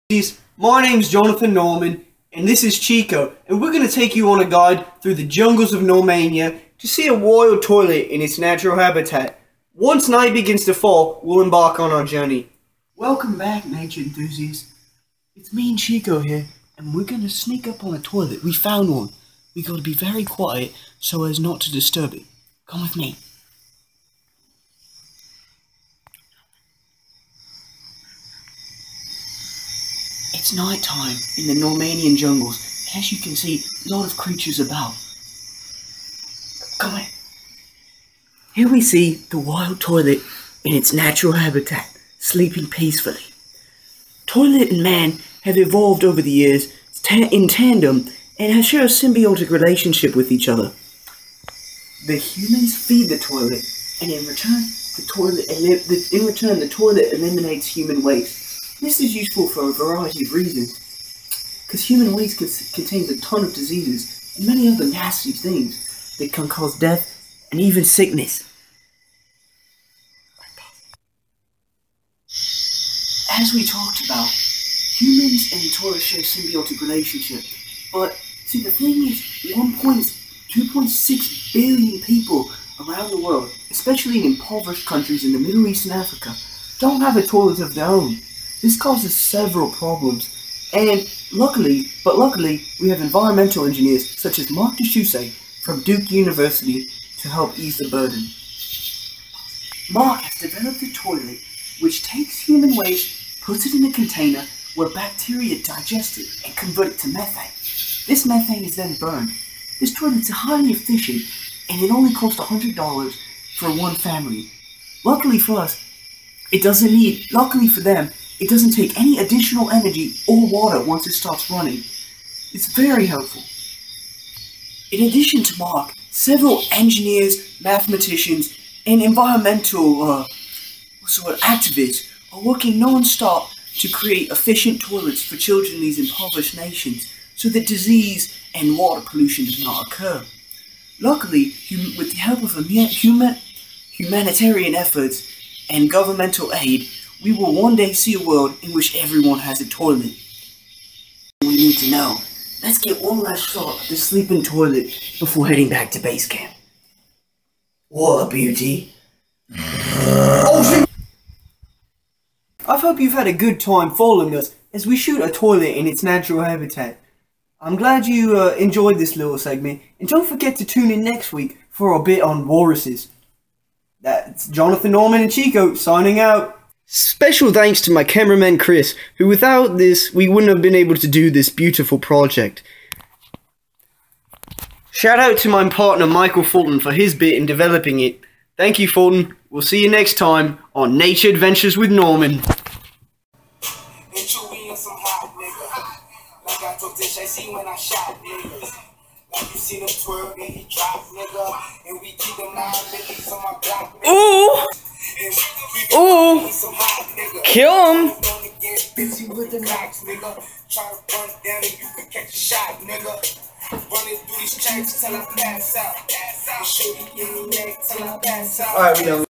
Toilet Documentary
This week on Normans in nature we go  deep in the jungles of Normania to study a toilet in its natural habitat.